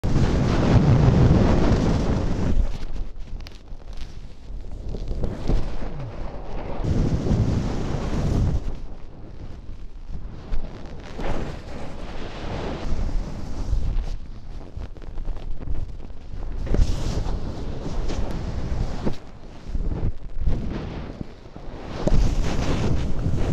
Four 540s
Four 540s with @GimbalGod

Shot in Saas Fee, Switzerland